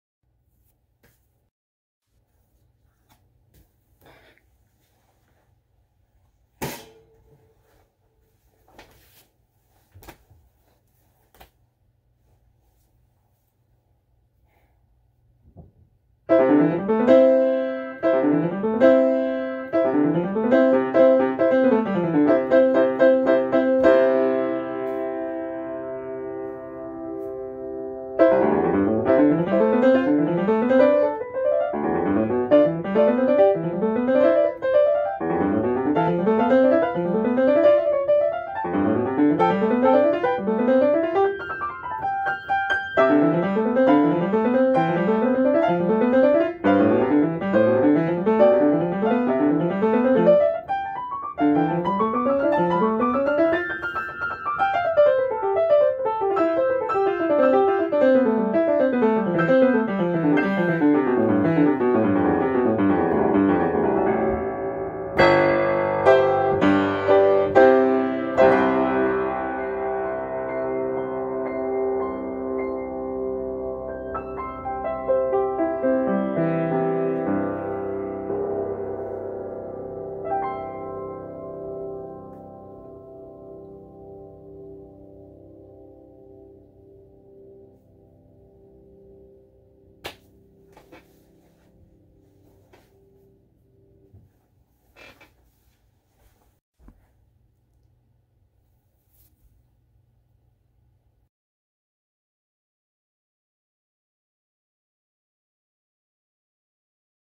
Czerny - (played not "too" imperfectly) - With added Intro and Ending parts composed by Me, Larry C.
Also showing my Director (Hailey Bailey) and my super un-fancy camera mounts.

1-1-2022